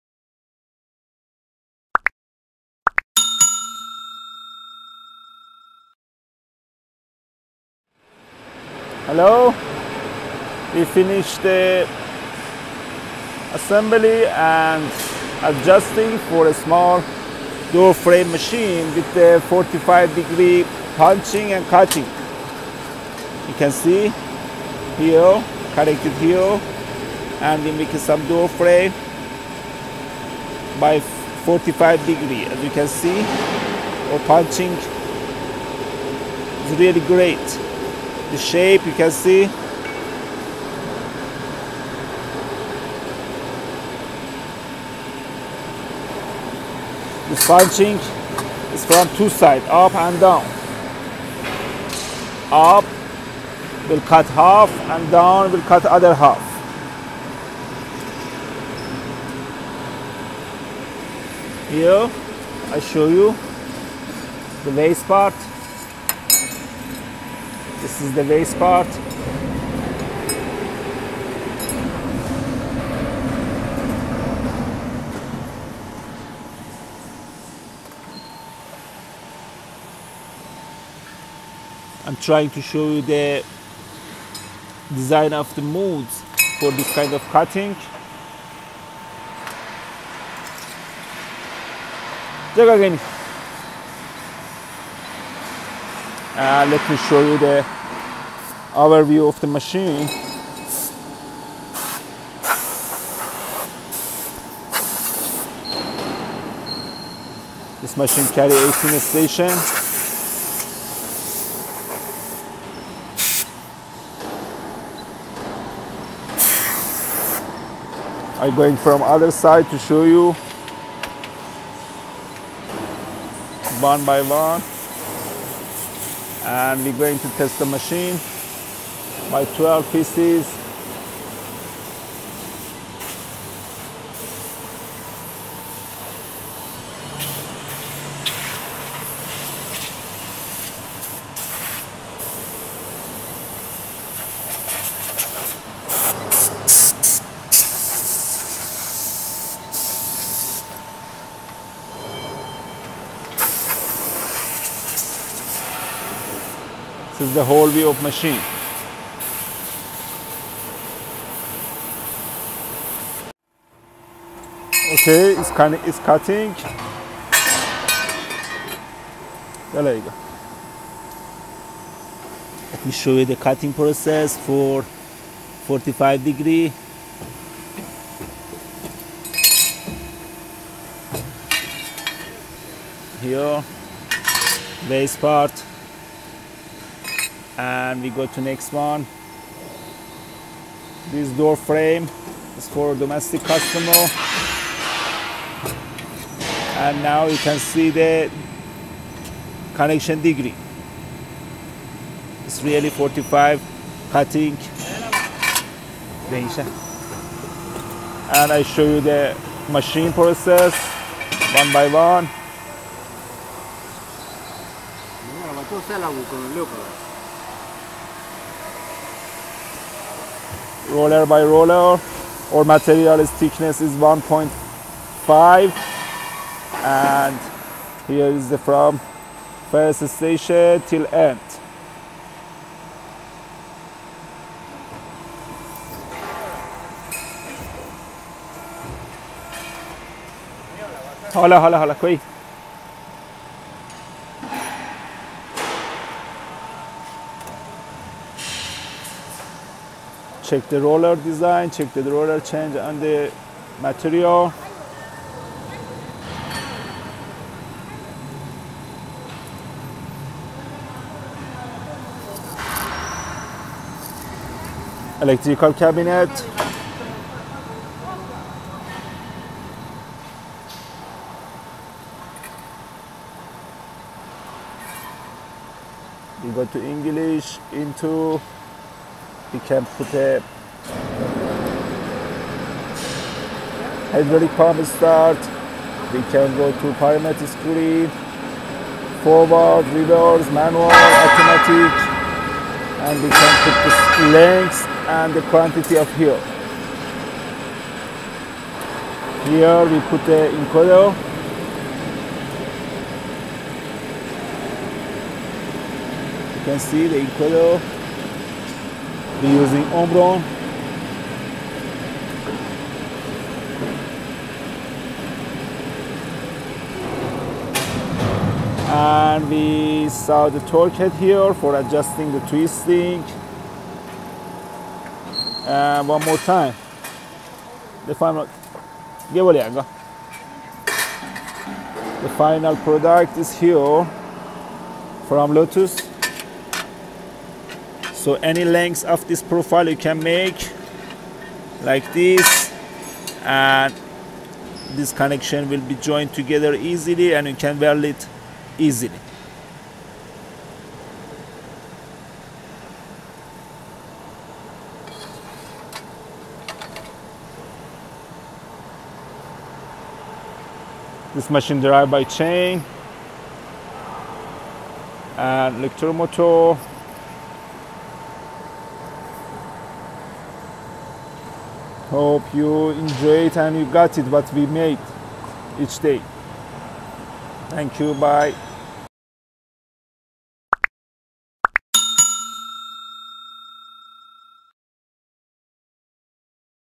Window door frame machine | Door frame 45-degree angle cutting machine " LOTOS DESIGN 2021"
Window door frame machine or Door frame 45-degree angle cutting machine can be customized as per customer requirement based that Mostly known for its stability and superiority.